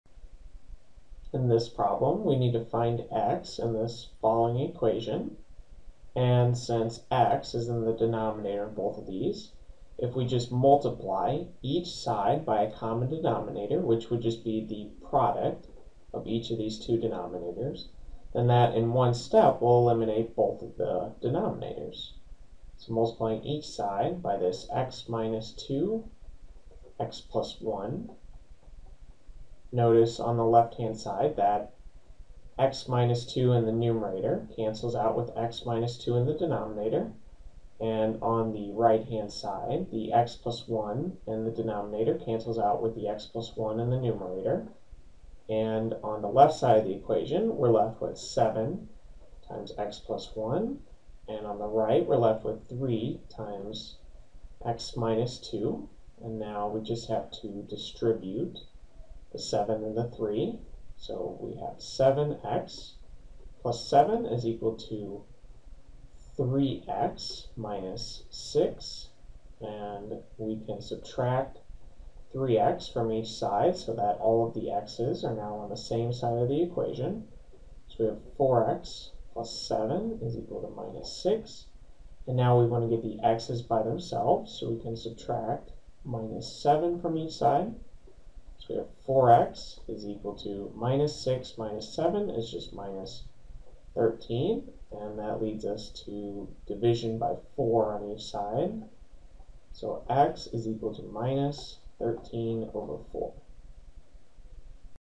CLEP College Algebra: Solving Equations 5
This is the forty third problem on the CLEP college algebra practice test